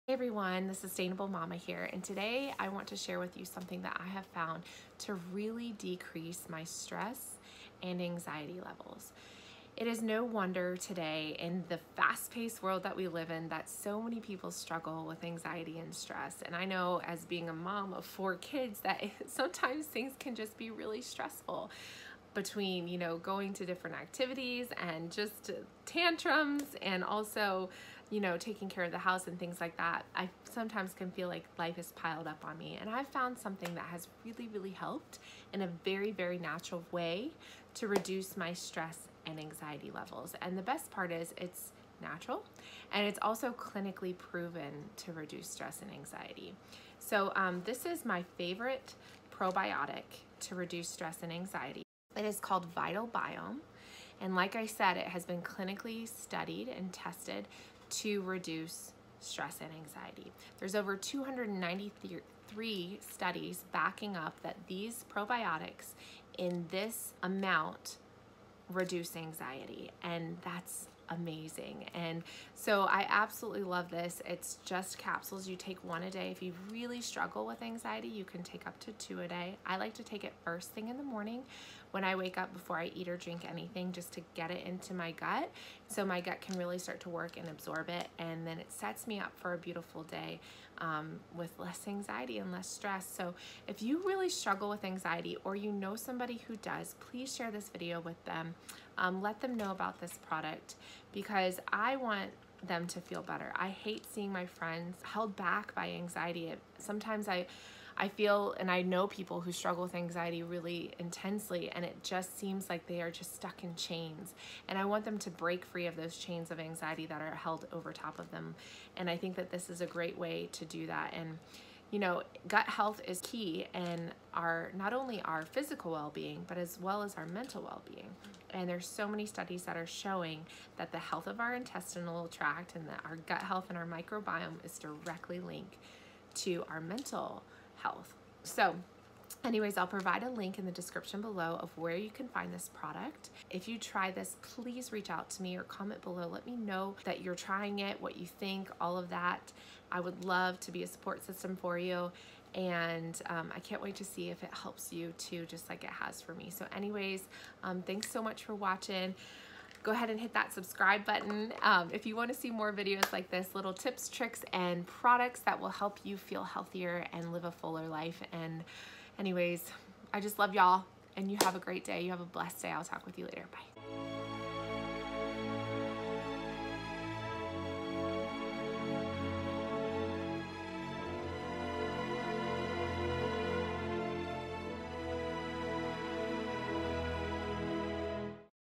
How to decrease anxiety and stress, naturally
So many people struggle with anxiety and stress and don't know where to turn for help.  The good news is that there are some amazing ways to decrease stress and anxiety naturally.  In this video I will show you one of my favorite ways to reduce stress and anxiety, naturally.

To try it for yourself with a 60 day satisfaction guarantee